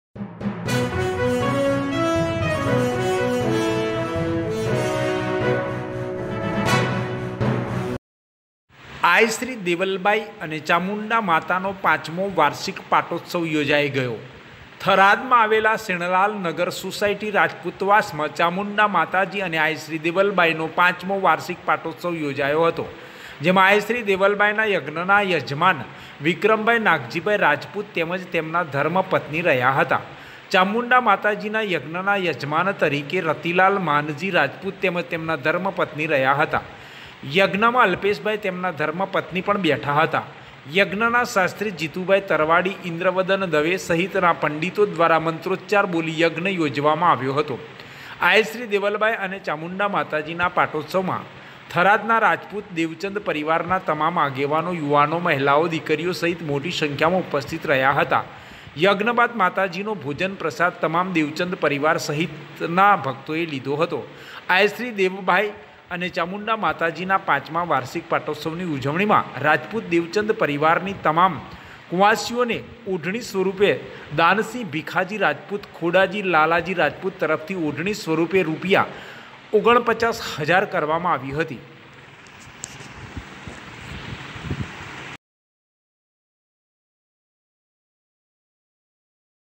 બનાસકાંઠા થરાદ રાજપુત દેવચંદ પરિવાર/આઈ શ્રી દેવલબાઇ અને ચામુંડ માતાનો પાંચમો વાર્ષિક પાટોત્સવ યોજાયો
𝗟𝗮𝘁𝗲𝘀𝘁 𝗡𝗲𝘄𝘀